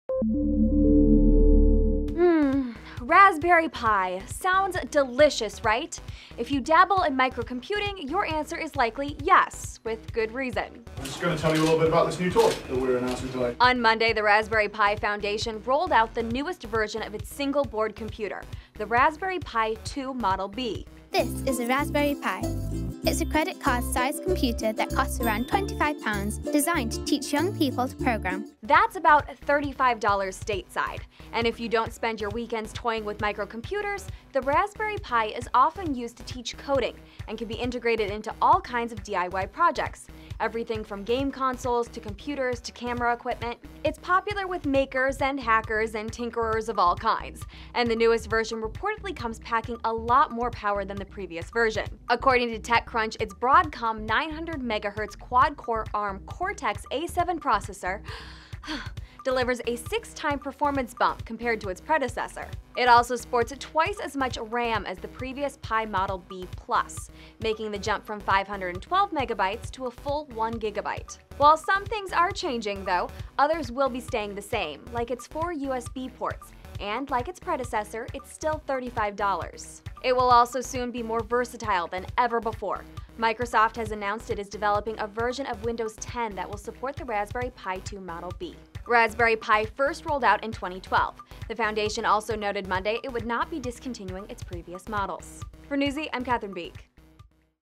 More Powerful Raspberry Pi Is Here, Still $35
The Raspberry Pi Foundation rolled out the newest version of its tiny single-board computer, the Raspberry Pi 2 Model B, Monday.